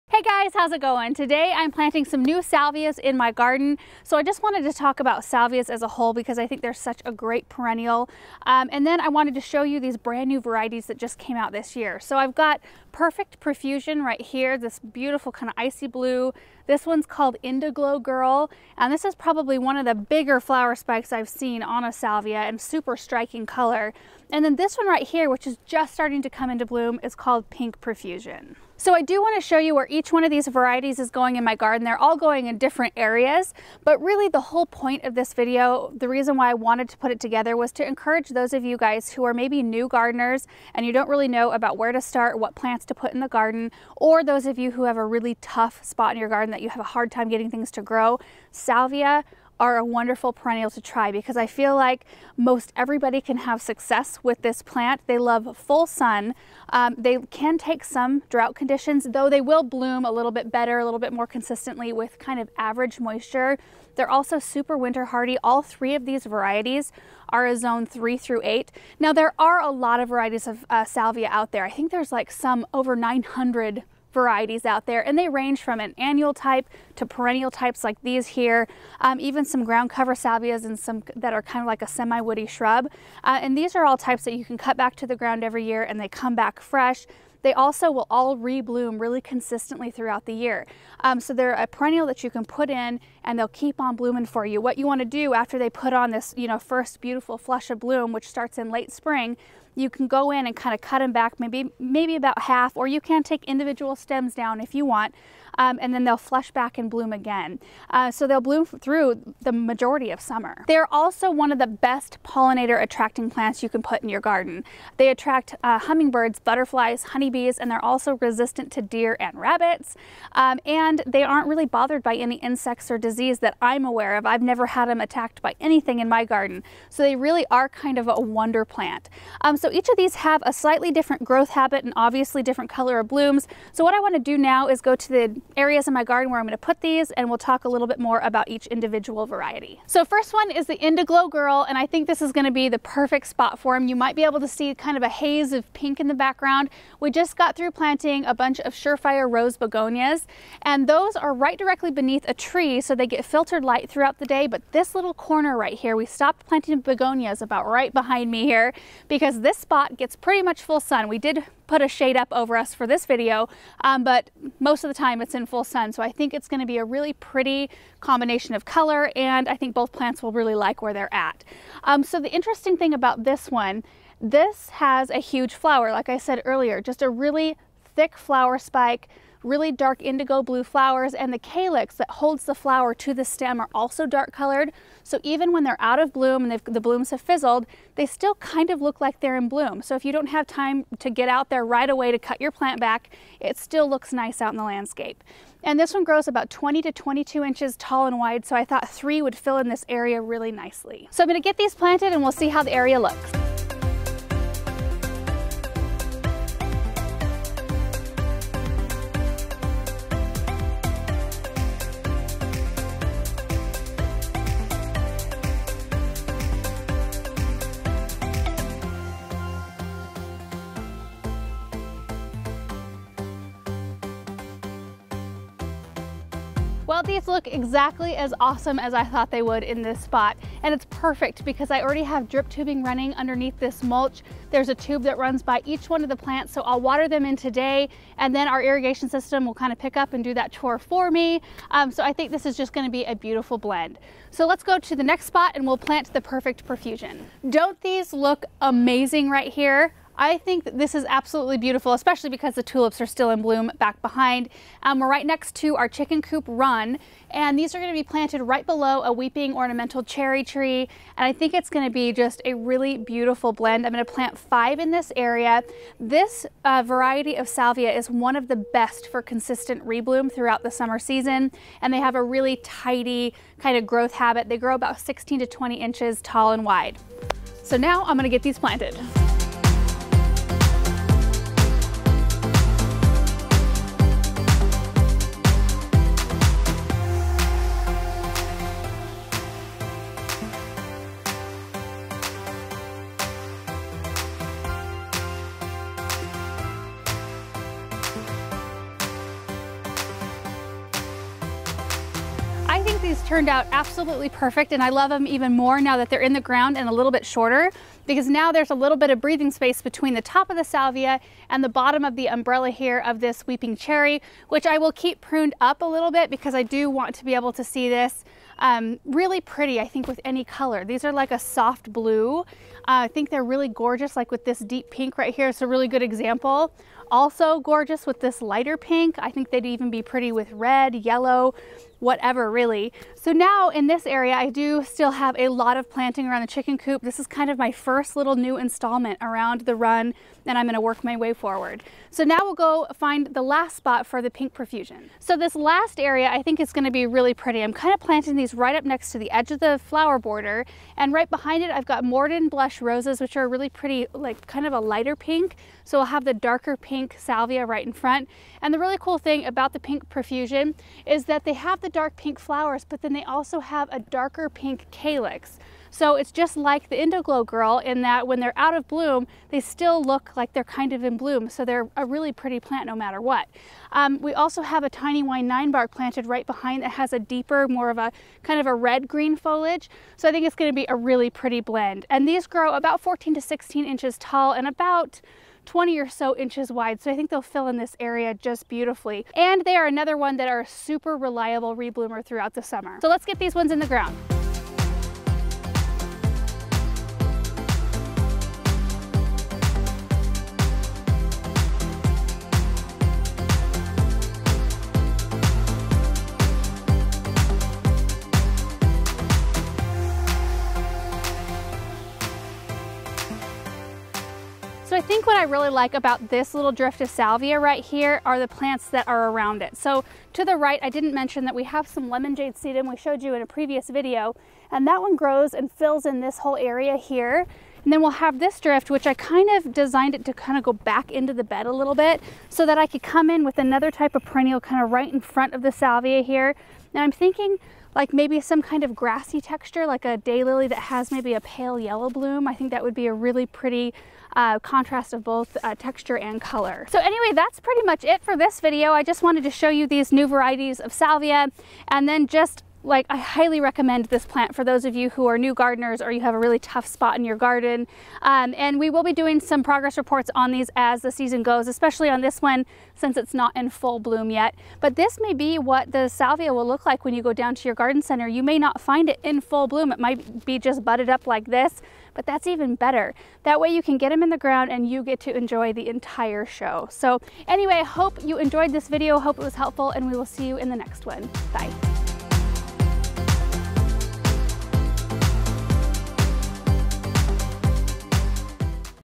Enjoying Perennial Salvia Plants in your Garden
You, along with the butterflies, bees and hummingbirds can enjoy these reblooming perennials all summer long. And, the deer and bunnies tend to walk on by--leaving the beautiful blooms for you! A light trim of spent blooms will have these plants gracing your garden with color all season.

FOLLOW US FOR EVEN MORE IDEAS AND GARDEN ADVICE!